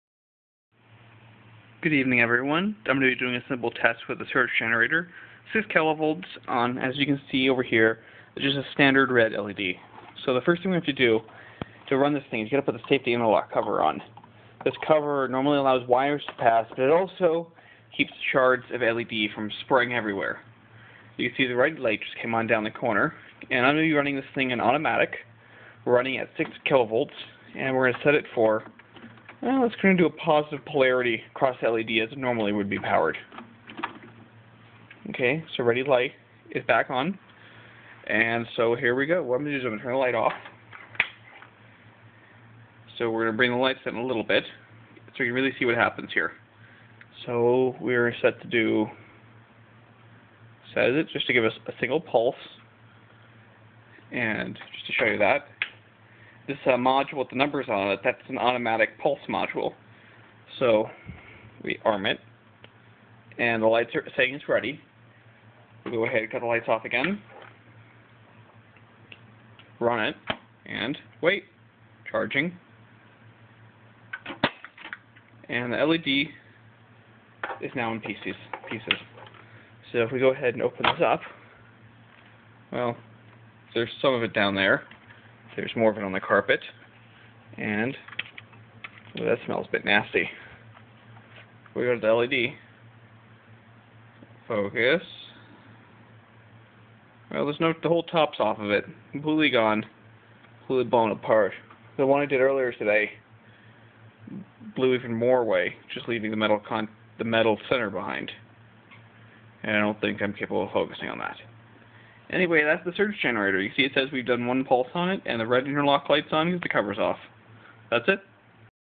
Blowing up an LED with a 6kV surge
Blowing up a red led using my surge generator. Unit is set to fire a single surge at 6kV max power.